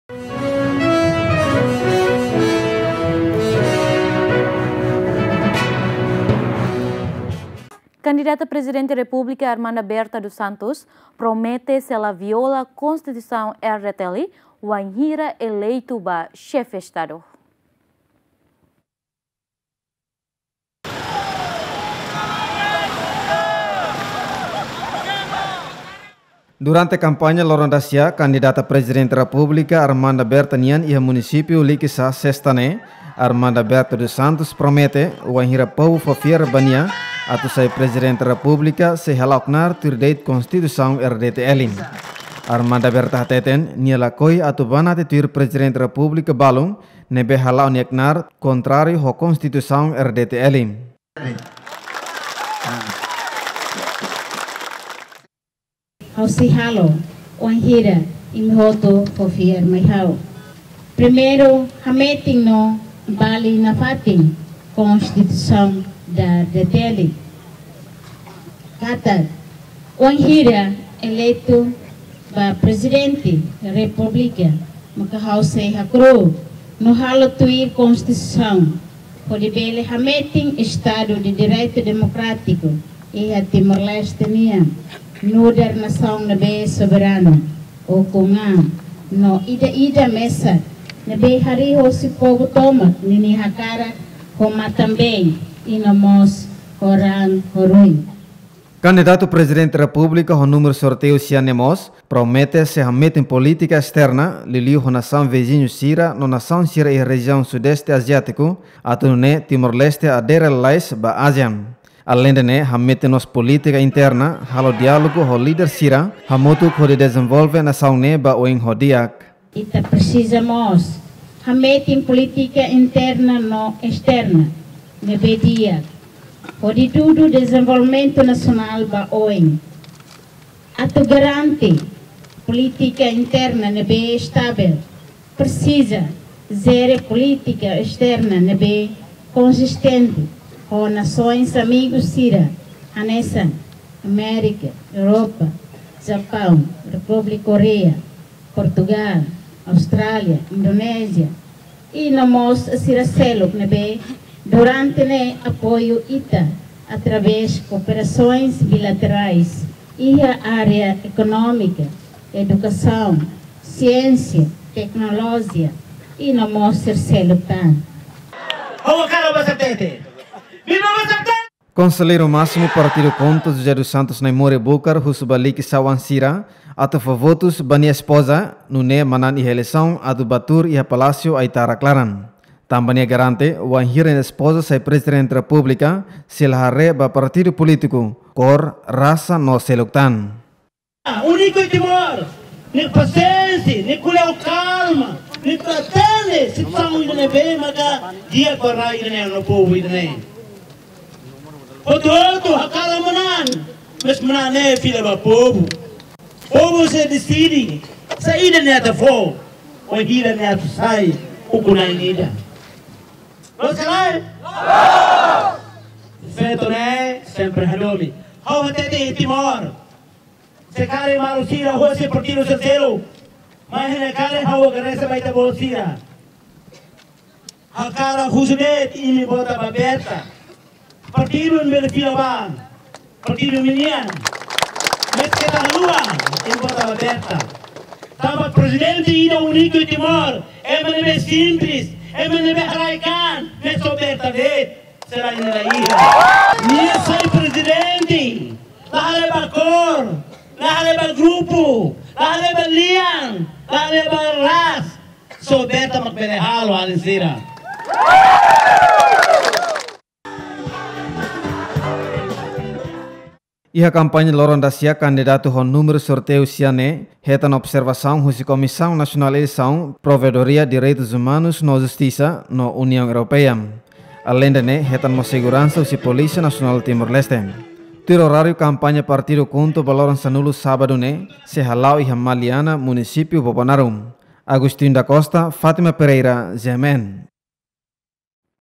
Armanda Berta promete hametin polítika importante rua, bainhira eleitu ba PR
Armanda Berta dos Santos promete hametin polítika importante rua, bainhira eleitu ba PR